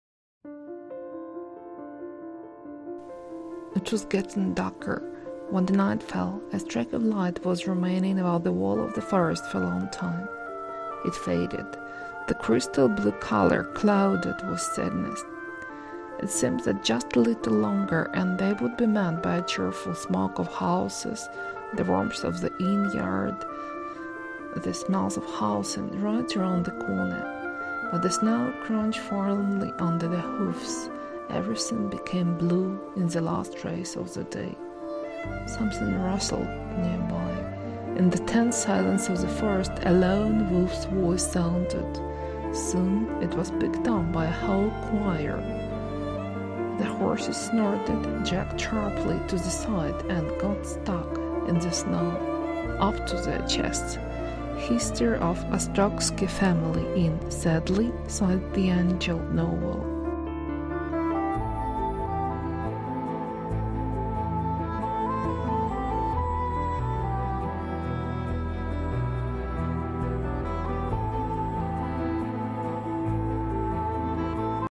Night in the winter wood. SADLY SIGHED THE ANGEL historical novel
Сутеніло. Коли опустилася ніч, над стіною лісу ще довго трималася світла смуга — вона тьмяніла, кришталево-блакитний колір затуманився смутком. Здавалося, що ще трохи і ось за тим поворотом їх зустріне веселий димок хат, тепло заїжджого двору, запахи житла. Але сиротливо похрускував сніг під копитами, в останніх променях дня все стало синім. Поруч щось зашурхотіло. У напруженій тиші лісу пролунав самотній голос вовка. Невдовзі його підхопив цілий хор. Коні схрапнули, різко смикнулись вбік і по груди загрузли в снігу.
It was getting  darker. When the night fell, a streak of light was remaining above the wall of the forest for a long time - it faded, the crystal blue color clouded with sadness.  It seemed that just a little longer and they would be met by the cheerful smoke of houses, the warmth of the inn yard, the smells of housing right around the corner.  But the snow crunched forlornly under the hooves,  everything became blue in the last rays of the day.  Something rustled nearby.  In the tense silence of the forest, a lone wolf's voice sounded.   Soon it was picked up by a whole choir.  The horses snorted, jerked sharply to the side and got stuck in the snow up to their chests.